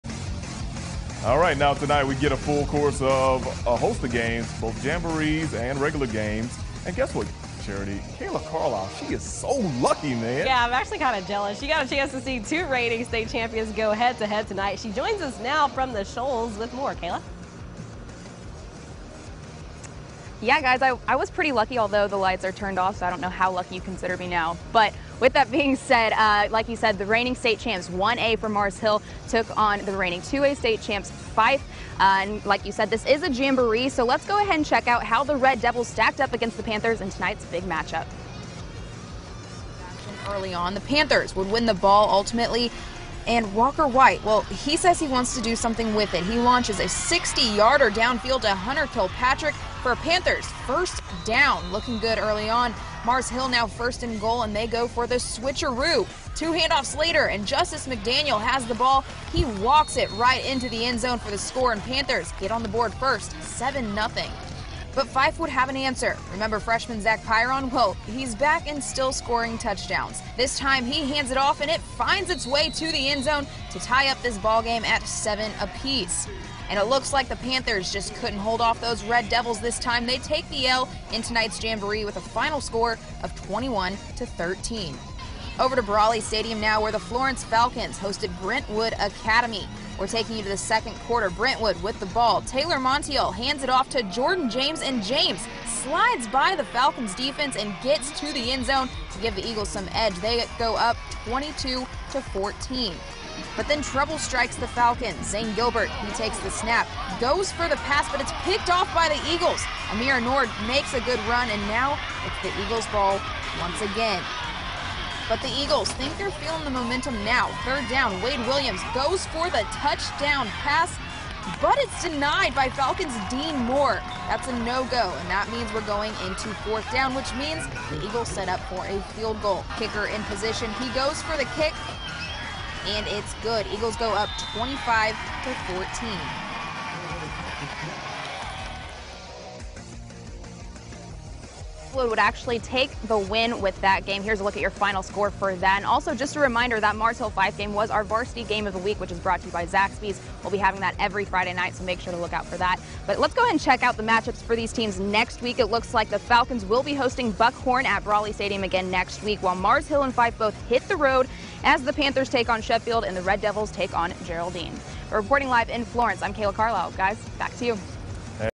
FDFN Varsity Game of the Week - Fyffe vs Mars Hill
The Fyffe Red Devils traveled to Florence to take on Mars Hill